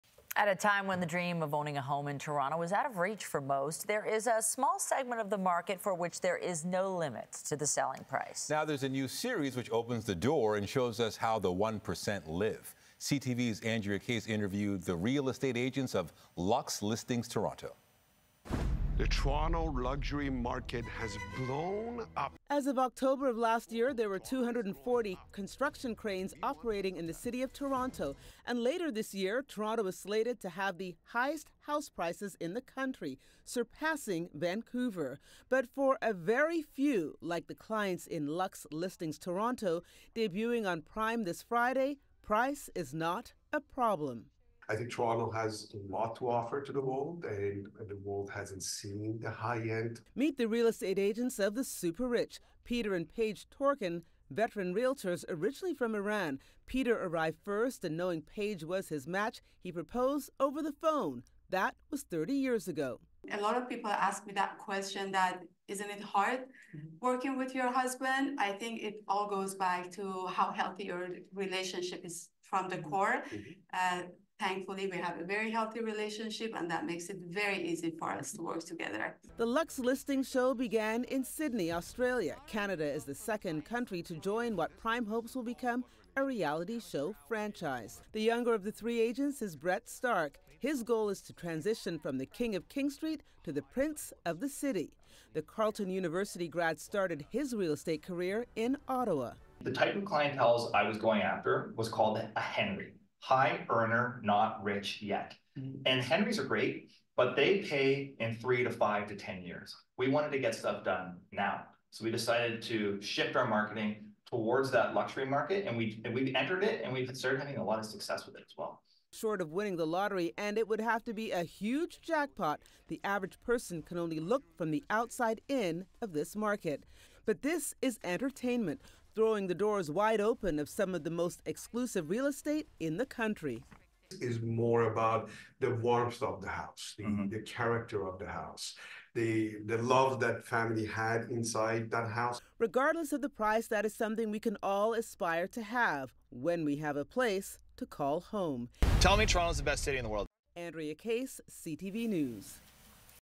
CTV News Interview with Luxe Listings Toronto Cast
Here is the full interview with Peter and Paige Torkan about Luxe Listings Toronto.